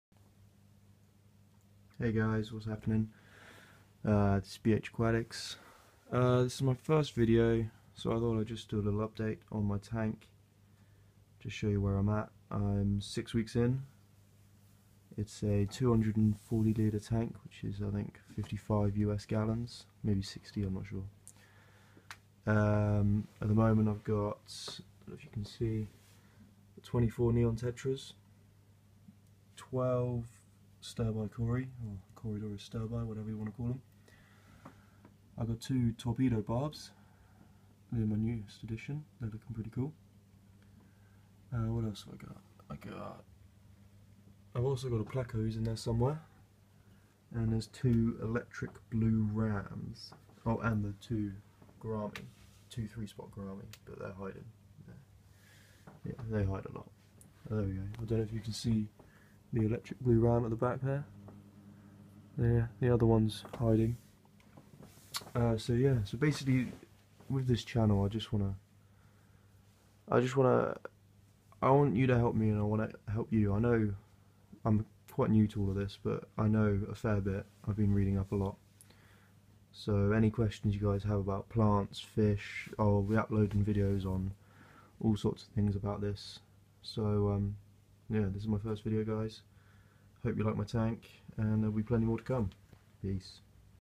First Video (Tank Update)
My first fish tank video. My tank is the Rio 240l (55g) and I've had it about 6 weeks. Its just fully cycled so i thought id make a video on it now. hope you like my tank guys and don't forget to subscribe as there will loads of videos to come !
Stock List:
24x Neon Tetra
12x Corydoras Sterbai
2x Denison Barb (Torpedo Barb)
1x Common Pleco
2x Eletric Blue Ram
2x 3 Spot Gourami (Blue Gourami)